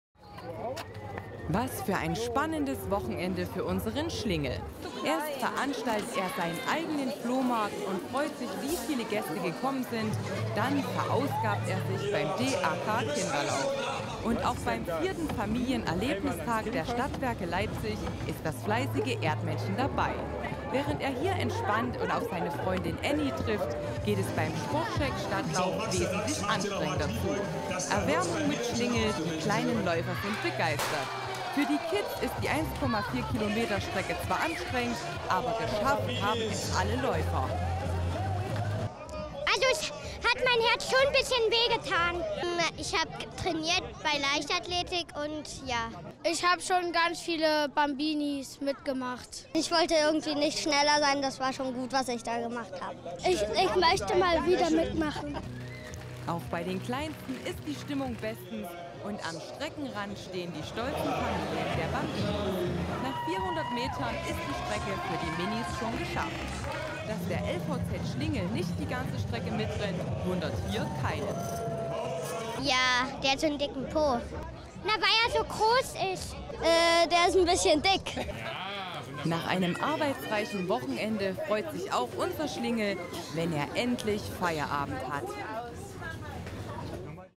Arbeitsreiches Wochenende für den LVZ-Schlingel
Leipzig, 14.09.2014: Der LVZ-Schlingel hat immer viel zutun. Dieses Wochenende hat er einen Flohmarkt organisiert, war beim SportScheck Stadtlauf und hat den Familienerlebnistag der Stadtwerke Leipzig besucht.